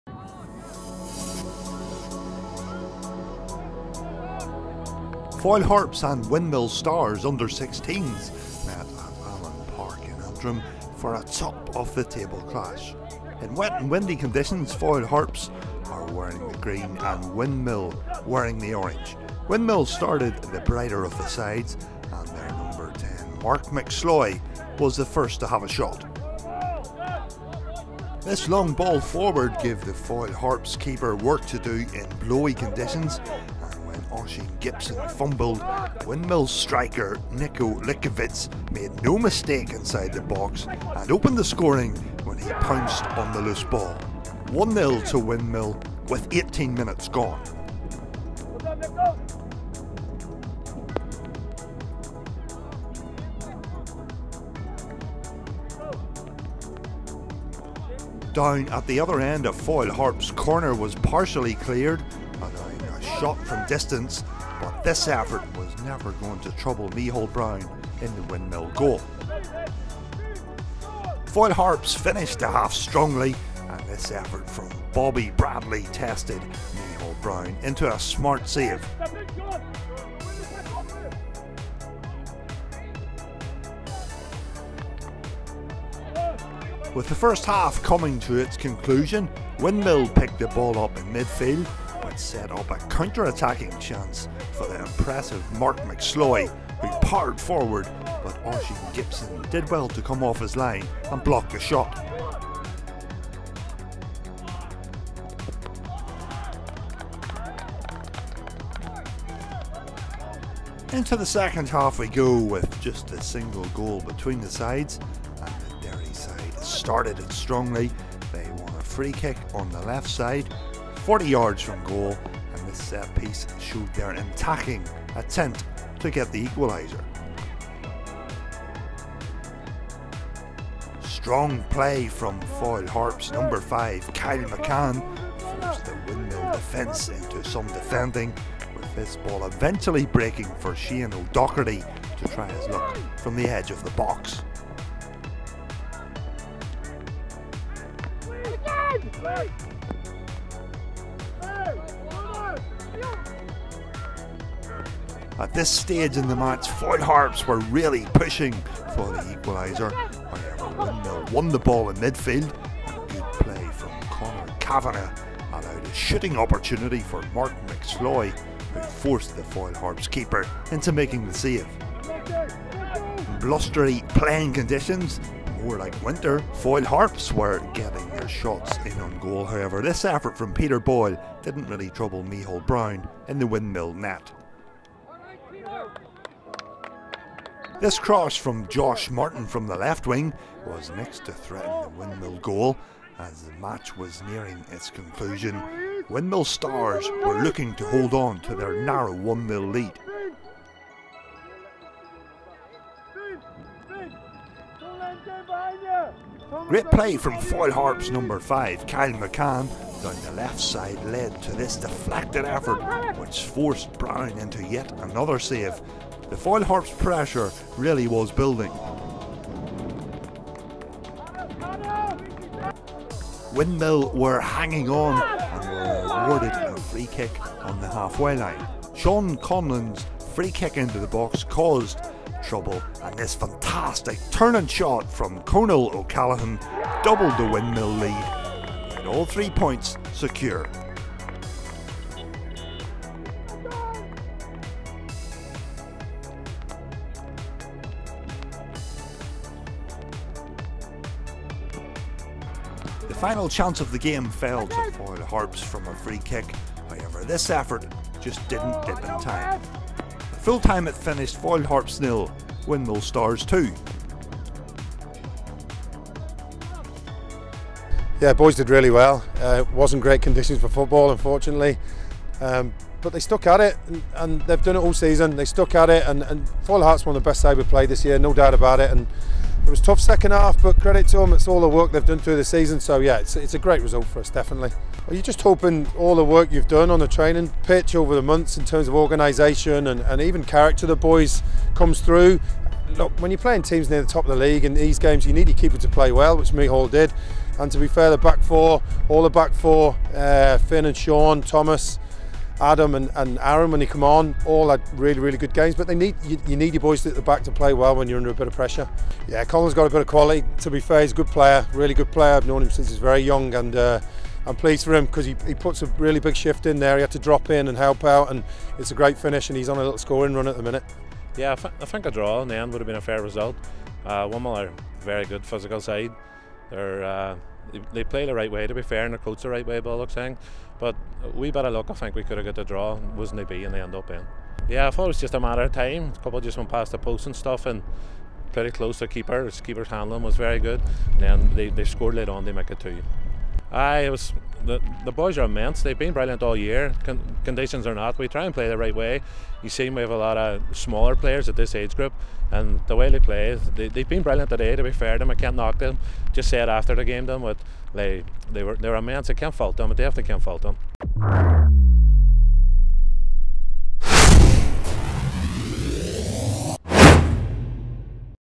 NIBFA TV (27/04/19)
Match report for Windmill Stars v Foyle Harps U16s